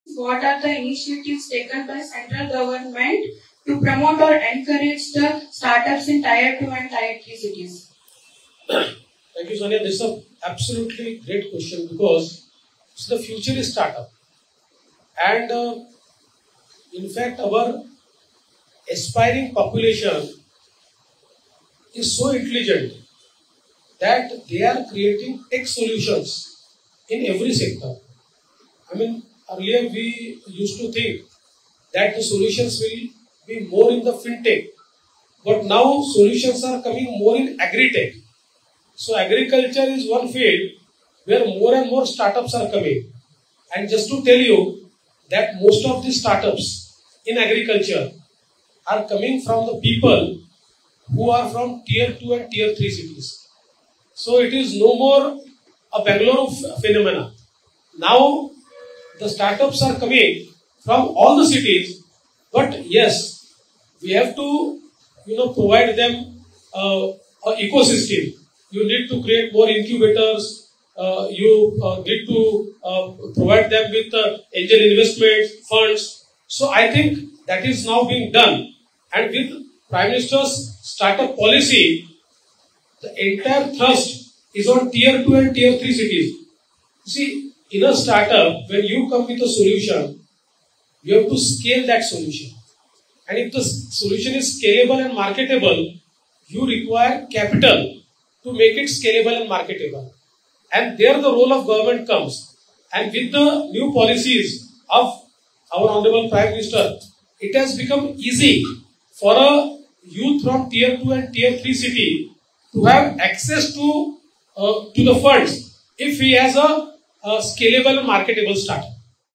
Most startups in Agri sector come from tier 2 and 3 cities |Dharwad,Karnataka| DCM Devendra Fadnavis
(Yuva Sammelan, Dharwad, Karnataka | 05/05/2023)